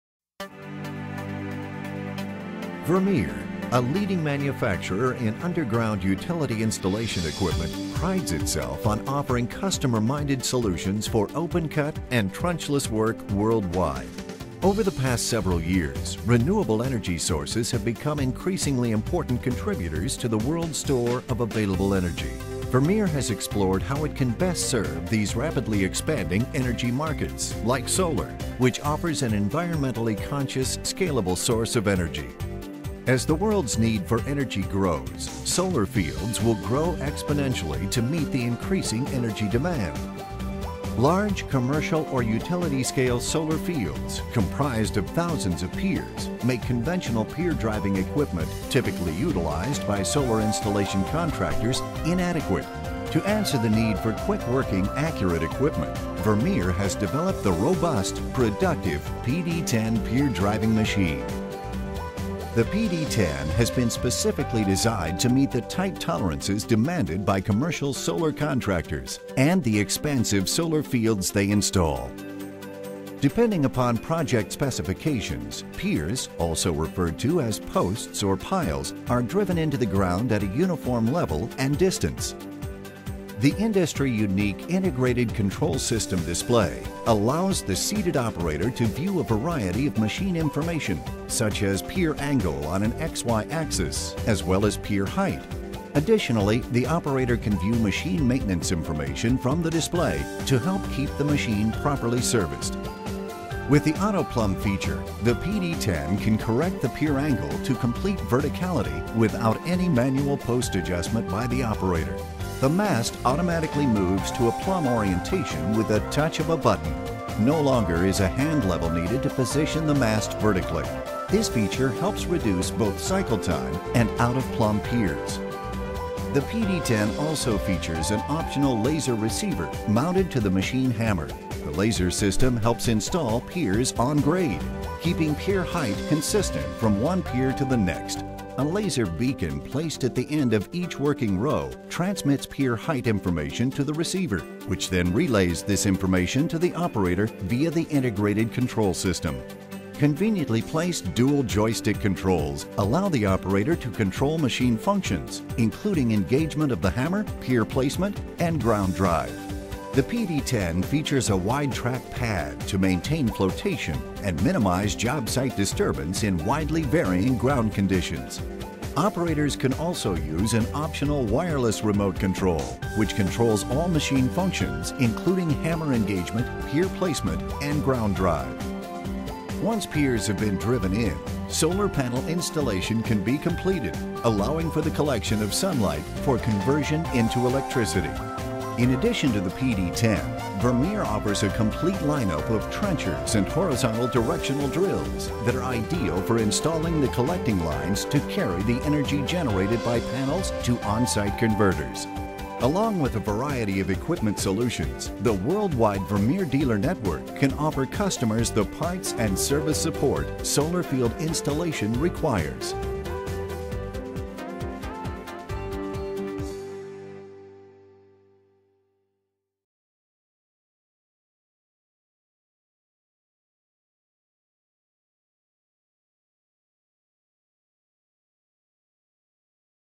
Vermeer PD10 Post Driver
To answer the solar industry's need for quick-working, accurate equipment, Vermeer has developed the productive PD10 pier driving machine, specifically designed to meet the tight tolerances demanded by commercial solar contractors and the expansive solar fields they install.